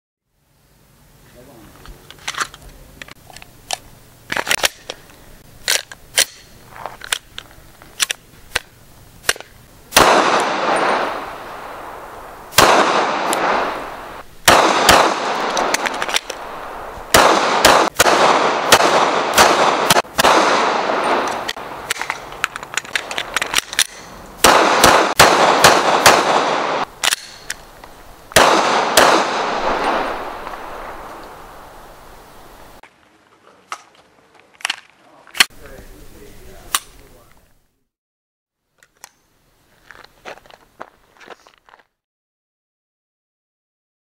Glock 19x Sound Effects
Una Glock 19x corredata di accessori TR-1upgrade in piccolo video in cui si possono sentire tutti gli effetti sonori durante la fase di caricamento e di sparo di questa fantastica pistola. Enjoy the Video and Sound!